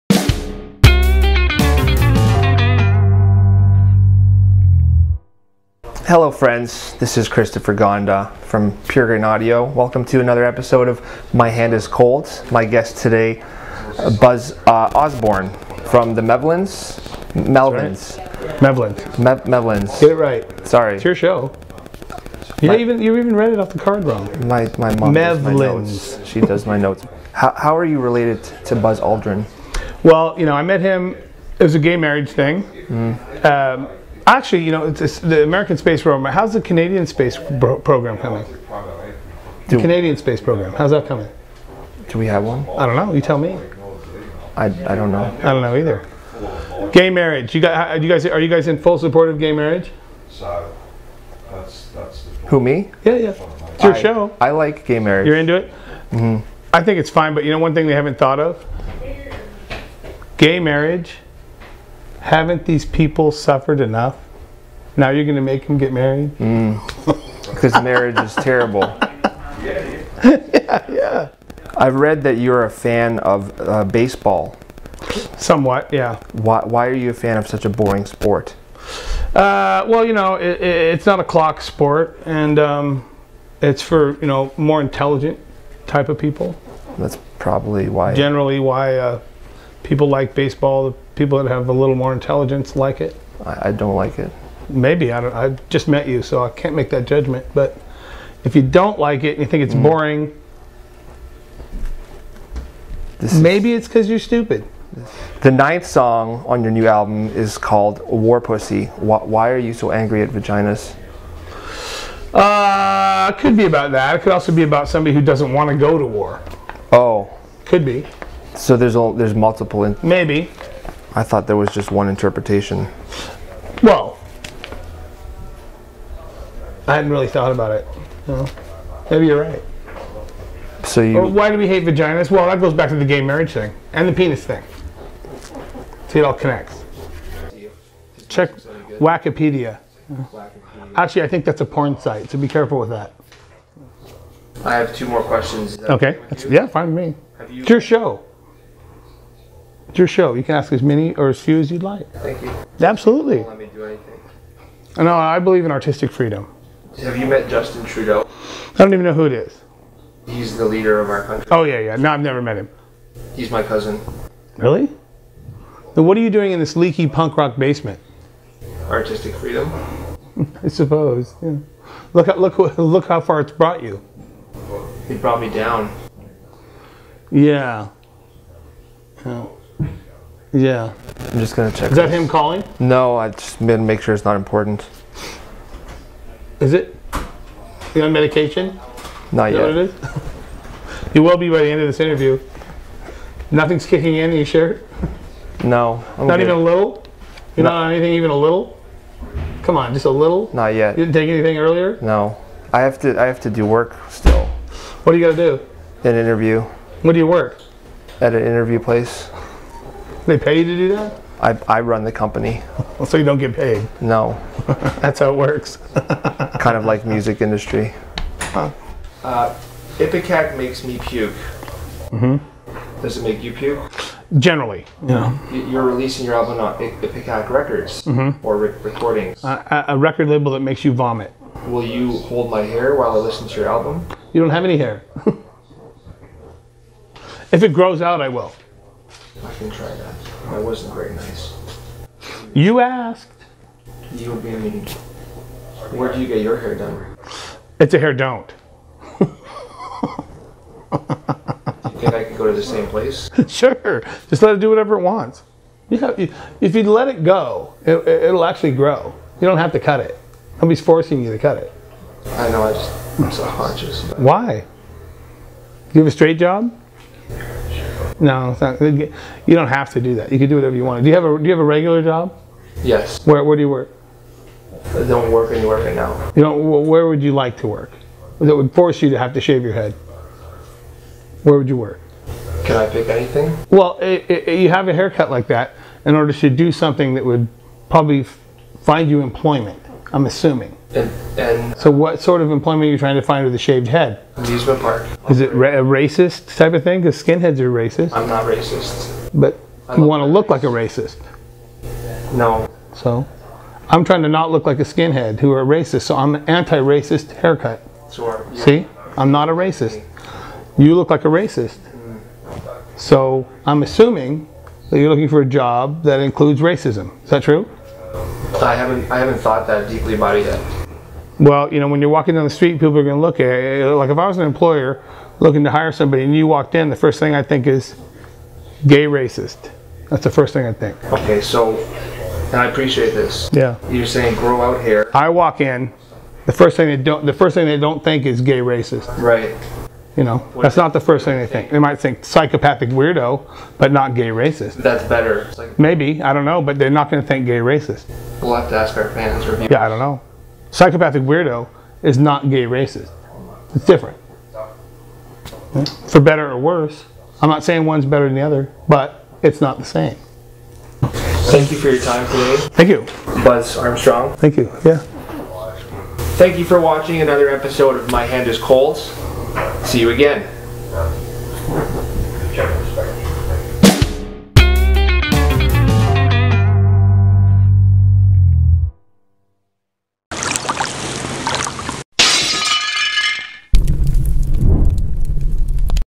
Melvins Interview on Racism, Gay Marriage and the Canadian Space Program - My Hand Is Cold #005
Iconic American band, Melvins, were in Toronto recently so we secured some interview time with singer and guitarist Buzz Osborne. We had a microphone fail on us but, thanks to the power of technology, managed to salvage our chat. In this episode of My Hand Is Cold, Buzz was kind enough to teach your host, Christopher Gonda, about gay marriage and instruct him on how to get a job. The two also discussed the Canadian space program, Prime Minister Justin Trudeau and puking.

Videography by: CGR Productions
Editing by: Brutally Delicious Productions
Video Series Treatment by: Marc Fourreau
Theme Song by: The Henry Maneuver